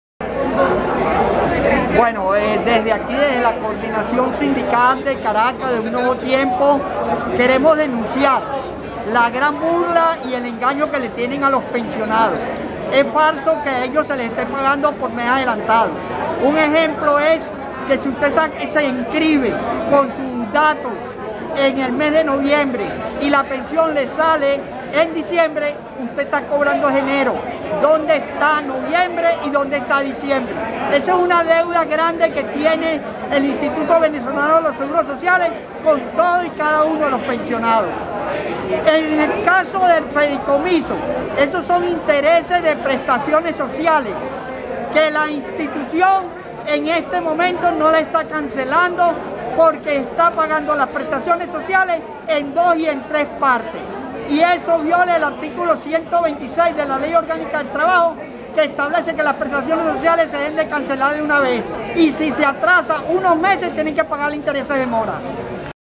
Carlos Julio Rivera:" Puras irregularidades y mentiras es lo que ofrecen"
El dirigente sindical jubilado del Instituto venezolano  del  Seguro Social denuncian en torno a las pensiones y al Fideicomiso